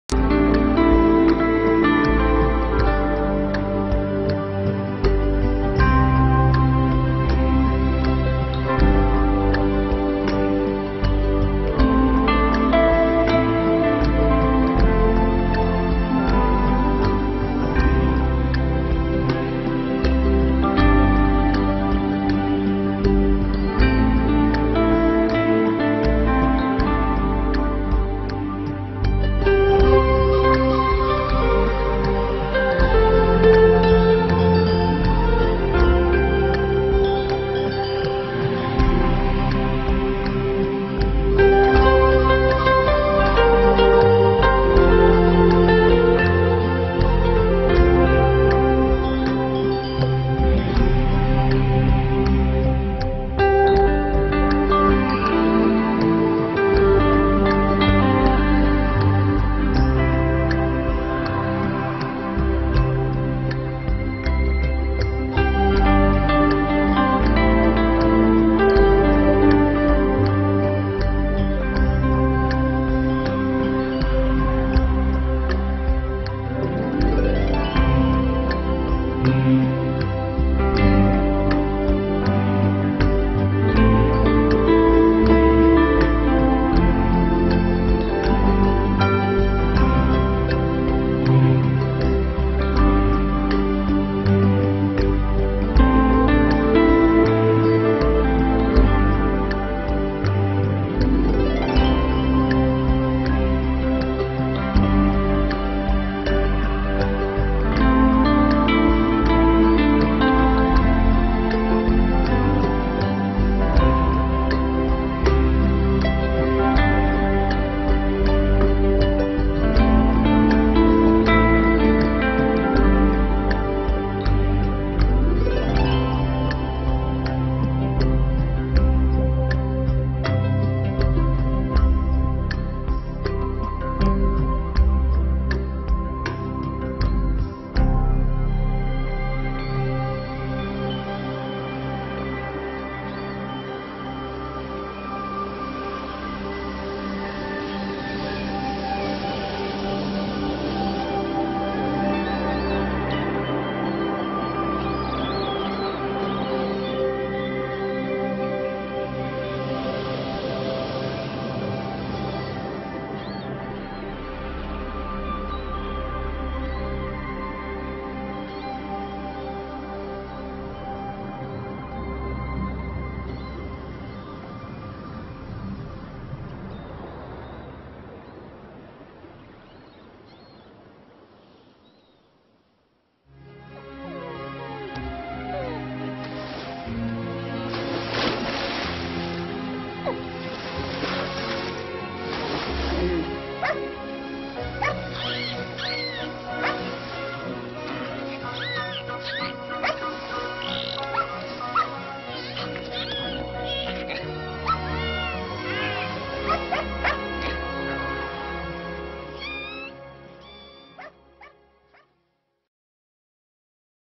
Amor en distintas especies
Este vídeo se muestra que el amor es inseparable hasta en ditintas especies